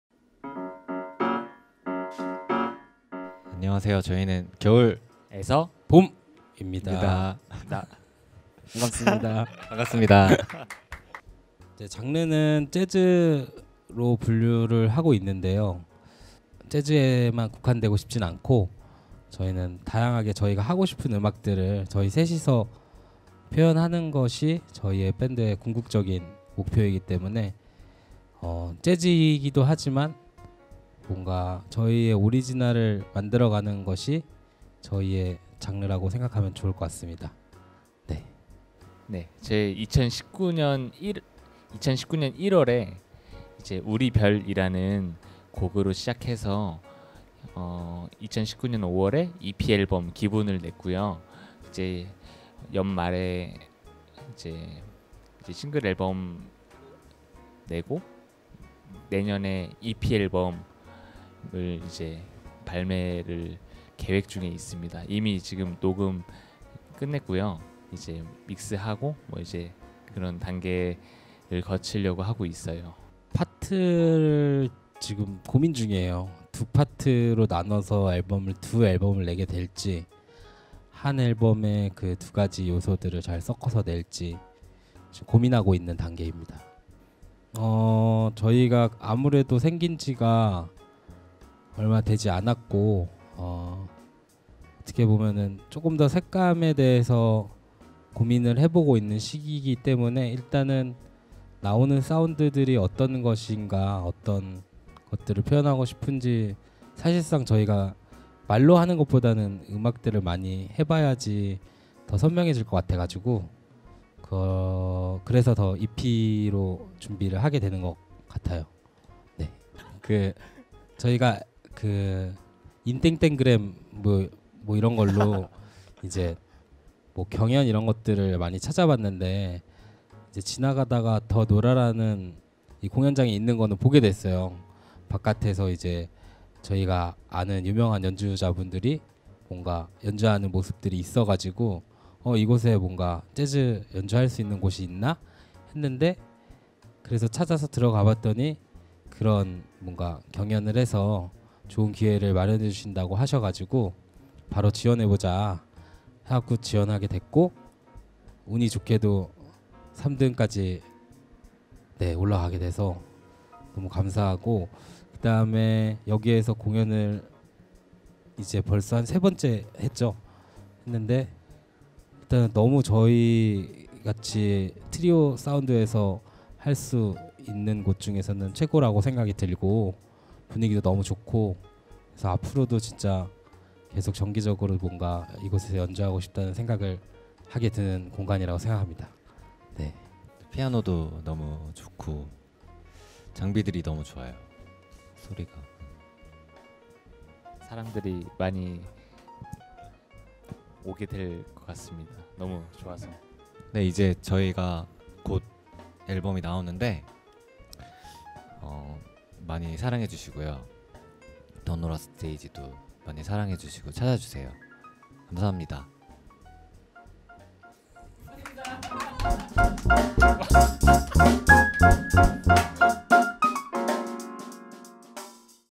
The Nora 더노라 [겨울에서 봄 - 인터뷰] 2019.11.08 FRI
더노라💕
[겨울에서 봄 & 블락스] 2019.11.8 FRI
'겨울에서 봄'은 피아노,베이스,드럼 구성의 트리오 밴드이다. 트리오 구성이지만 재즈를 넘어 다양한 음악적인 시도를 하는 밴드이다.
House Engineer _ Doosol Antonio Kim
Recorded, Mixed by _ Doosol Antonio Kim
촬영/편집 _ AzzilhanKAM
더노라는 재즈,힙합,인디,팝,발라드등
다양한 장르의 음악공연이 펼쳐집니다.
더노라는 이도문화재단이 운영하는 홍대 라이브홀 입니다.